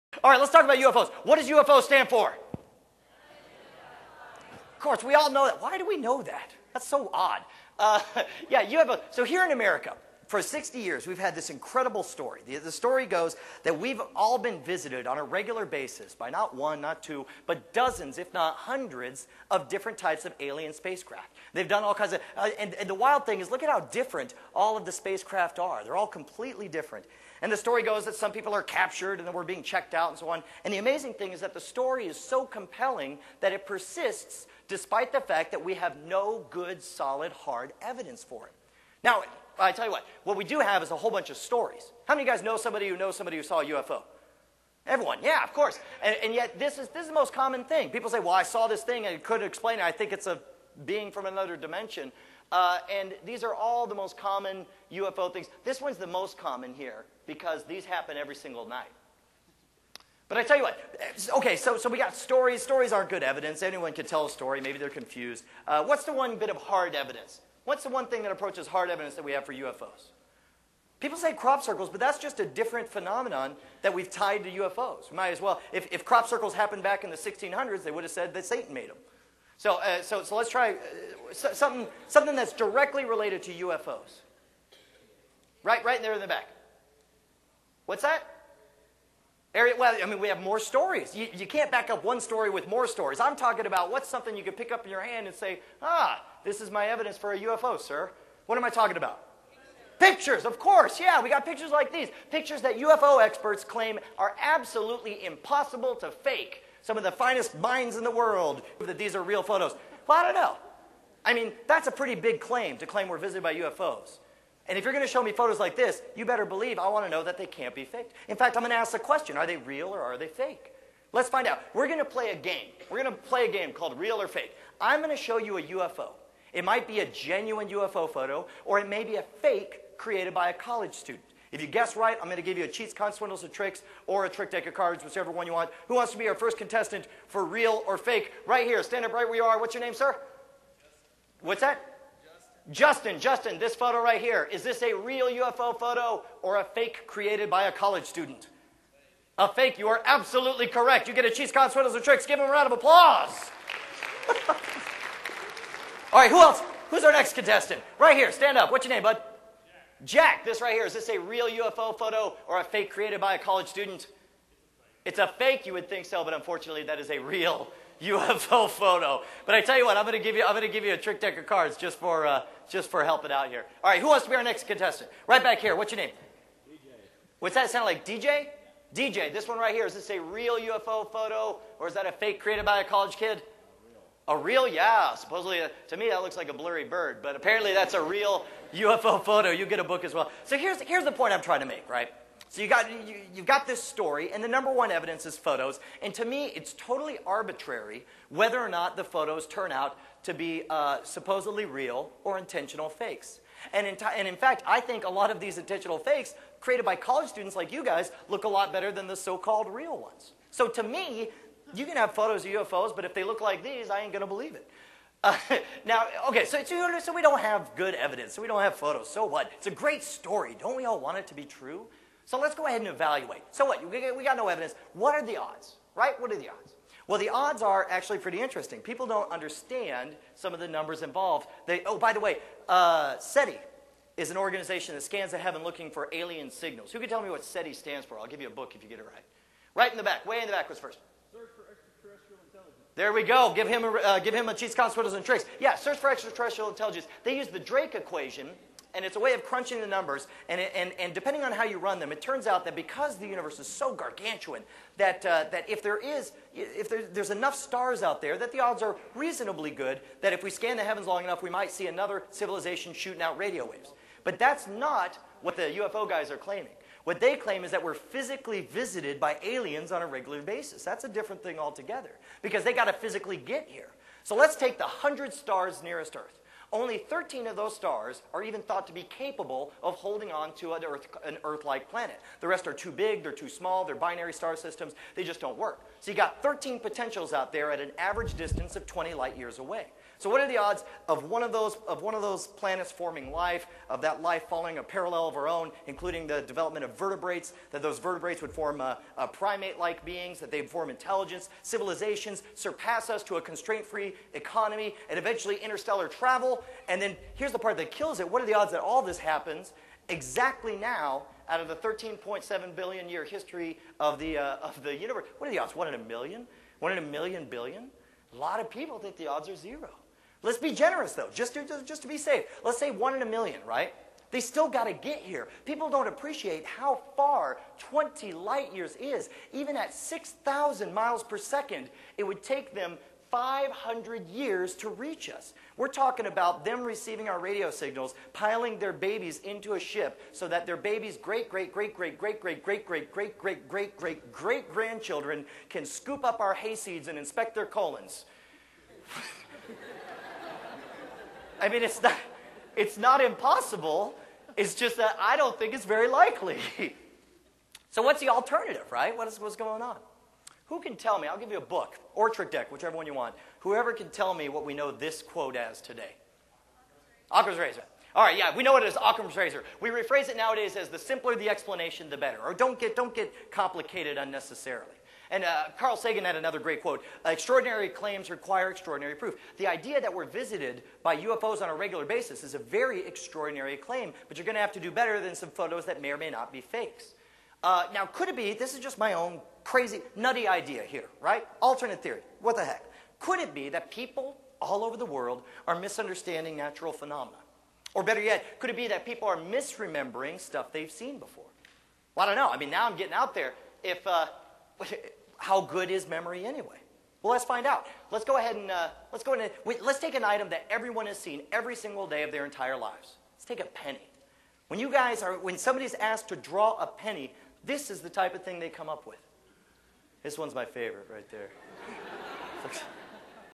SSS (4of14) UFO's... what are the odds?
from Brian Brushwood's lecture, Scams, Sasquatch and the Supernatural.

Brian talks about discerning good evidence from bad with UFO's, explains occam's razor, then evaluates the odds that we're physically visited by aliens.

Learn More about Brian, his lecture, and his "bizarre magic show"